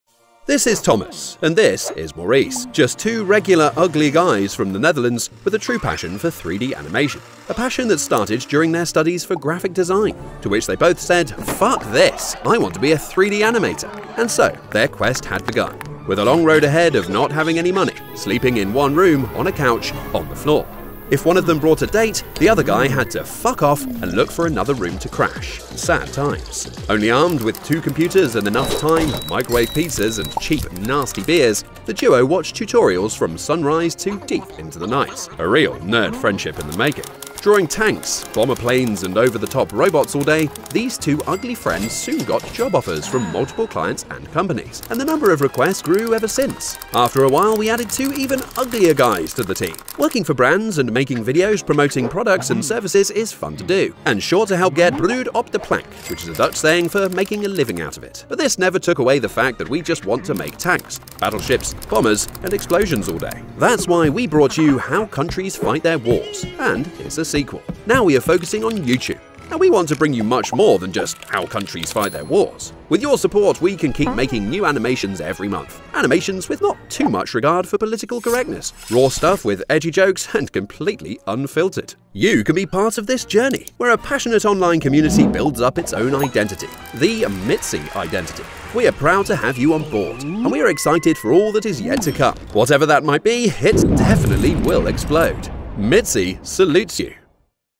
The Story Behind Mitsi.Studio
We are Thomas and Maurice, we are Mitsi Studio. We want to bring you cool animations about history, science, warfare, you name it!. Dive into the story behind Mitsi Studio!